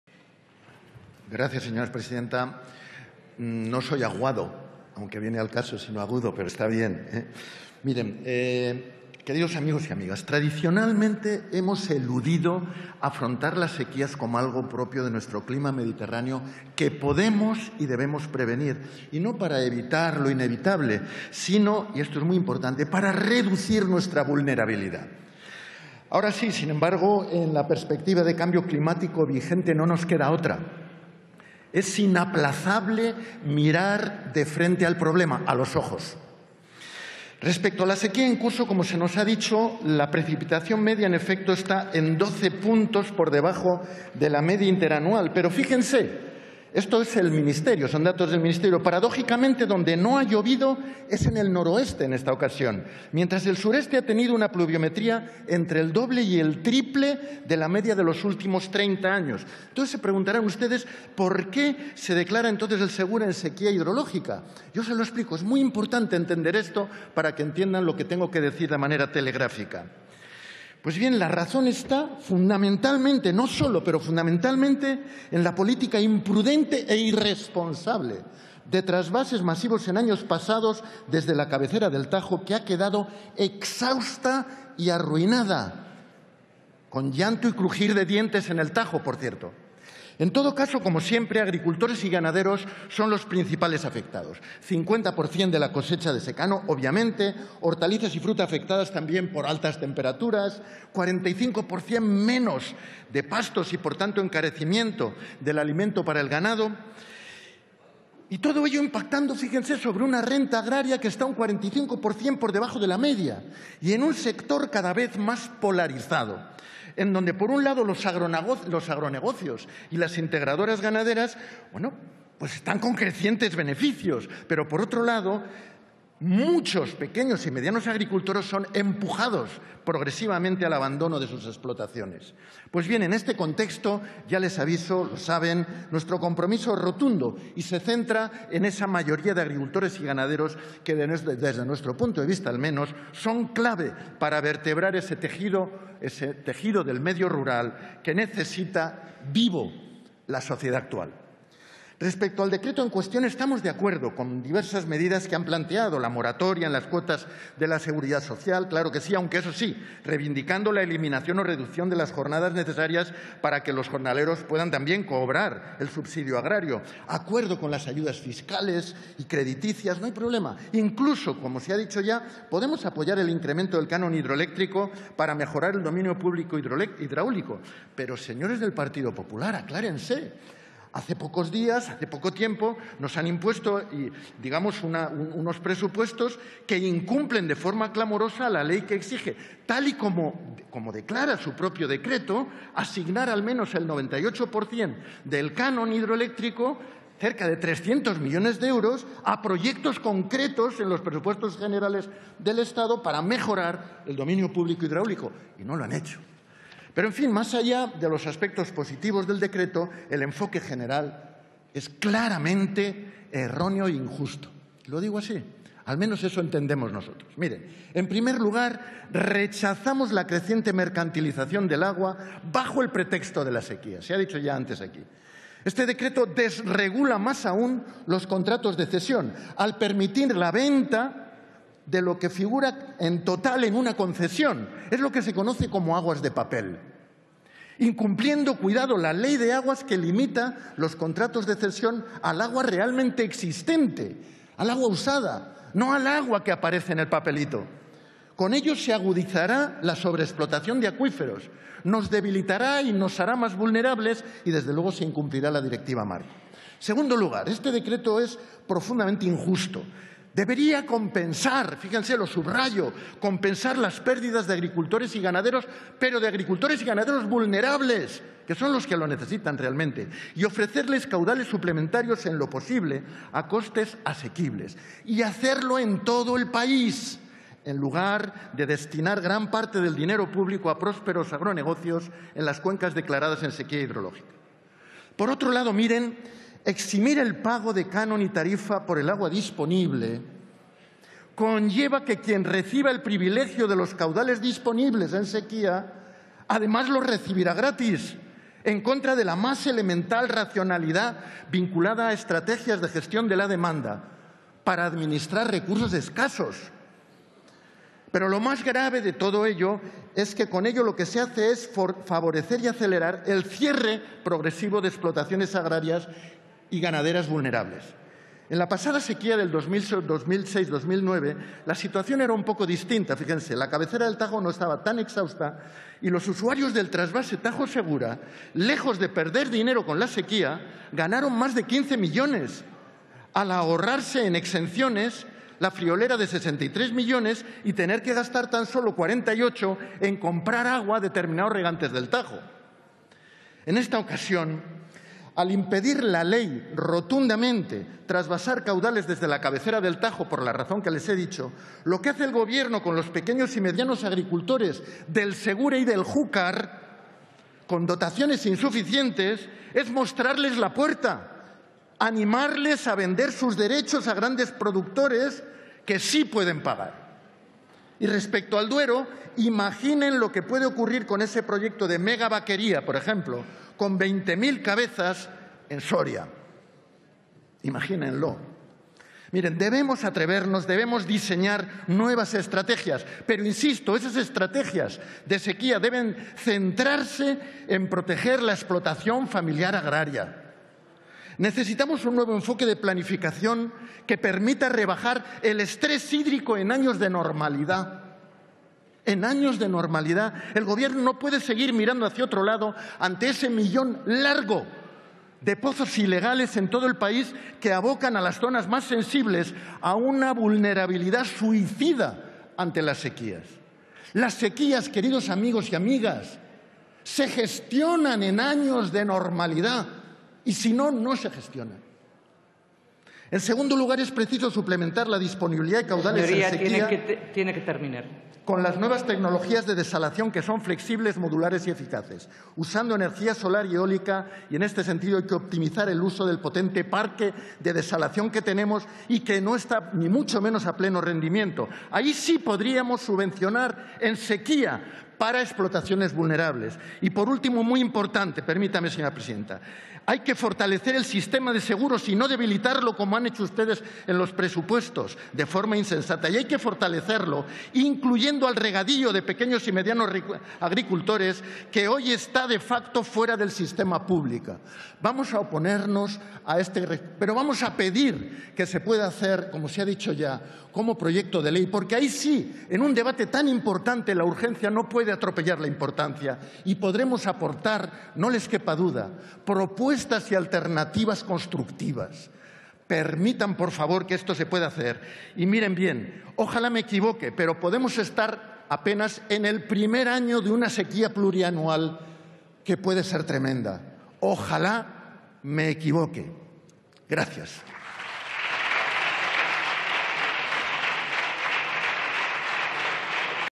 Rechazamos la creciente mercantilización del agua bajo el contexto de la sequía
Pedro Arrojo en el debate del Real Decreto-Ley 10/2017, de 9 de junio, por el que se adoptan medidas urgentes para paliar los efectos producidos por la sequía en determinadas cuencas hidrográficas y se modifica el texto refundido de la Ley de Aguas, aprobado por Real Decreto Legislativo 1/2001, de 20 de julio.